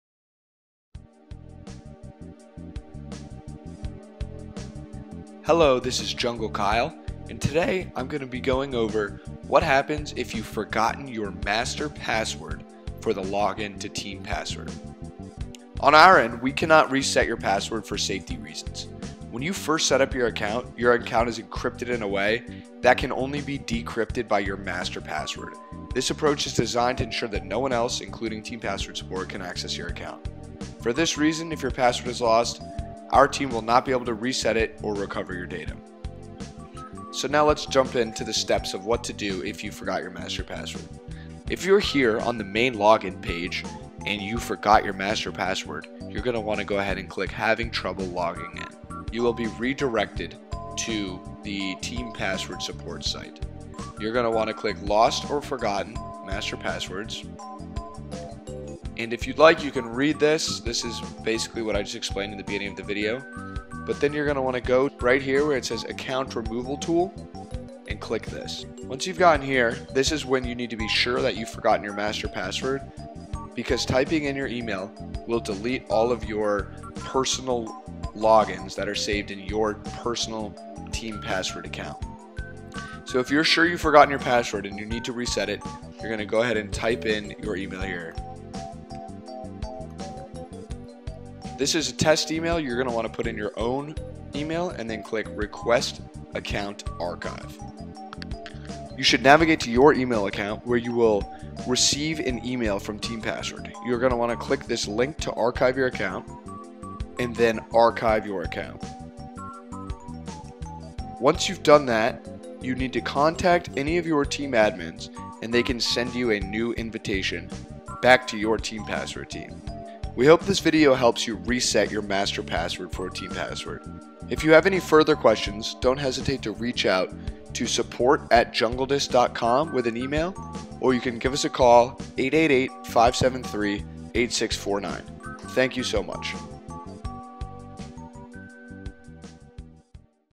TeamPassword: Lost or Forgotten Master Password
This video will walk through the instructions to help you reset your password.

Reminder: if you do this, you will lose your personal saved passwords.

Thank you for your time, enjoy the video.